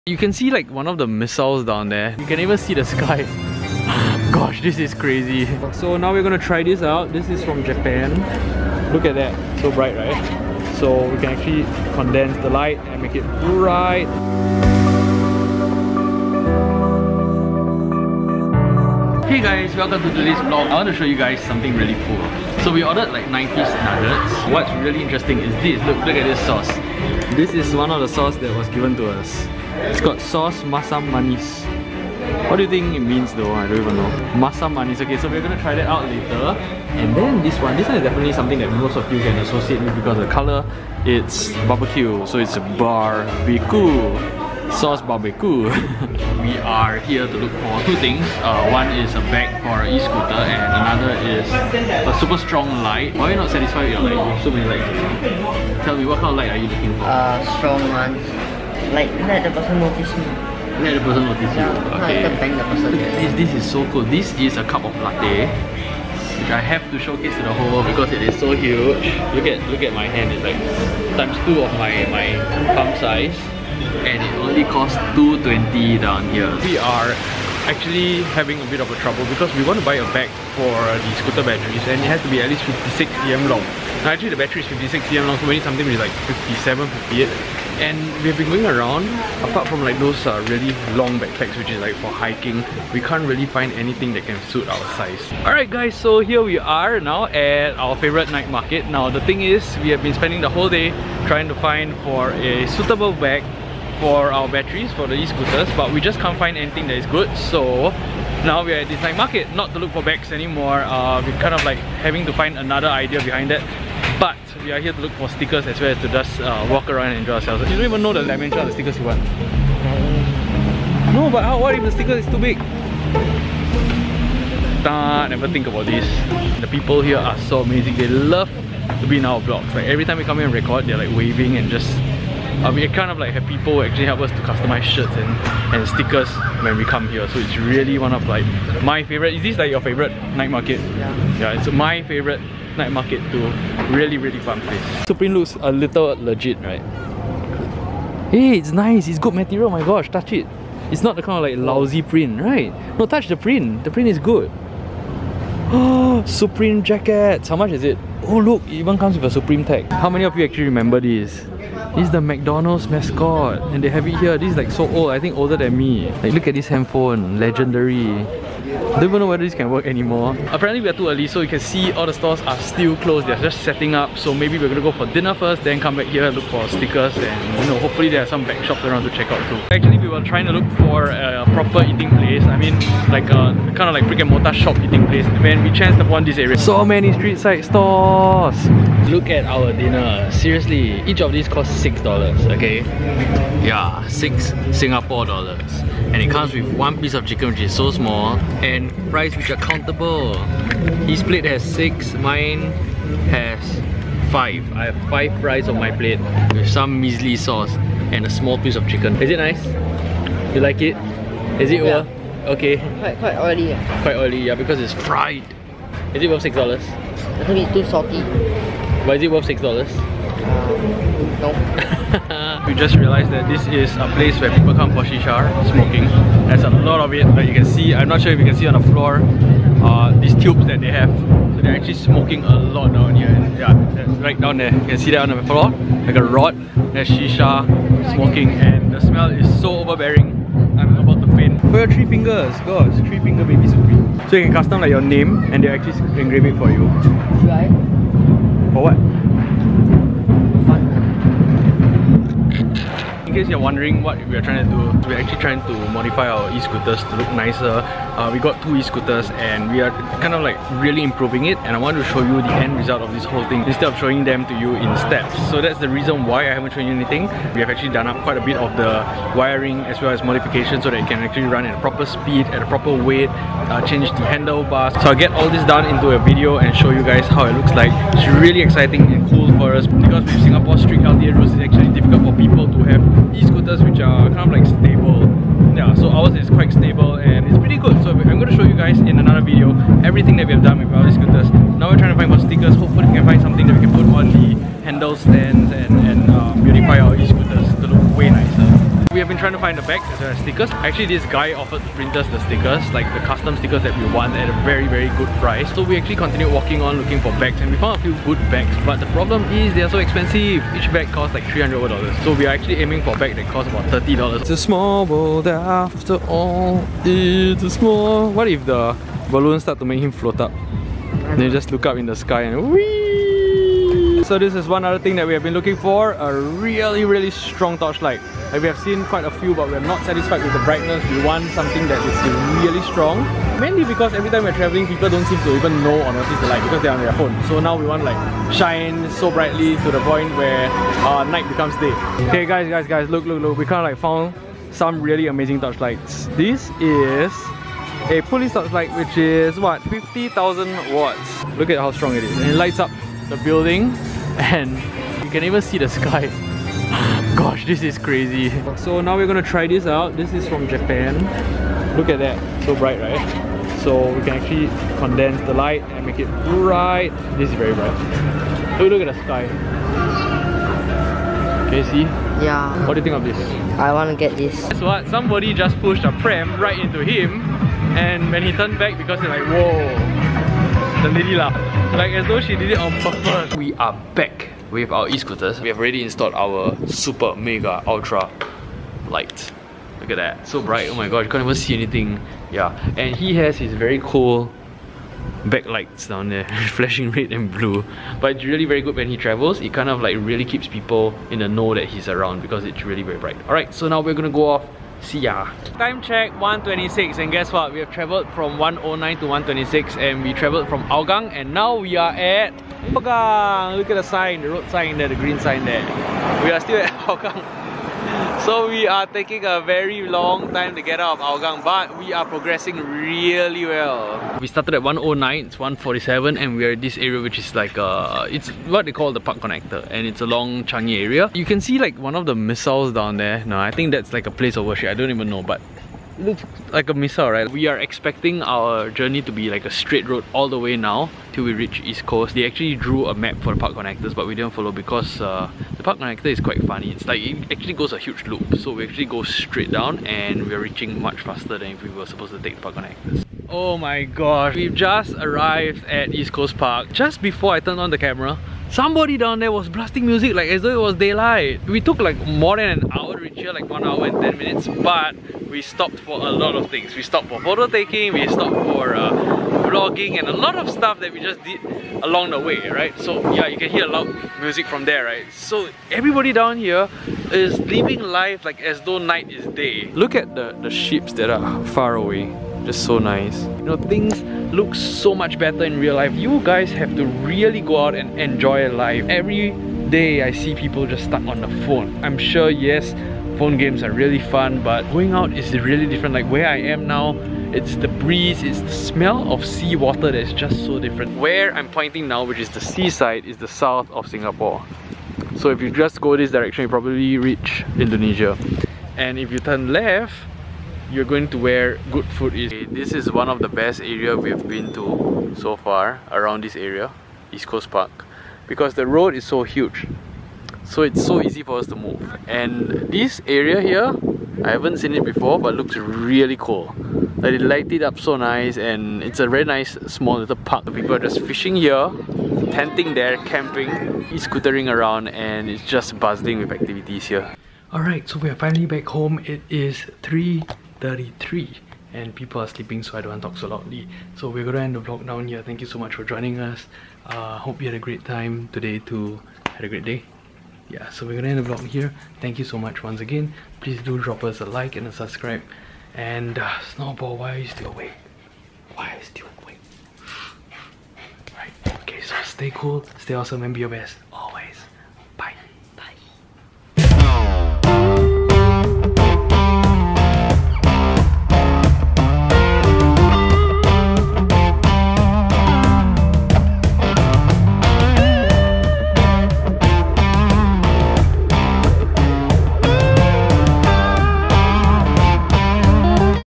NIGHT LIFE AT EAST COAST PARK, SINGAPORE
The night life at East Coast Park Singapore surprised me. We spent the day trying to modify our escooters for a travel to East Coast Park Singapore. Amazing sights as we travelled south. Finally reached East Coast Park, and the experience there was breathtaking.

For Mails, Gifts and Parcels, please send them to:
AStarVlogs
Blk 613
Hougang Ave 8
#02-446
Singapore 530613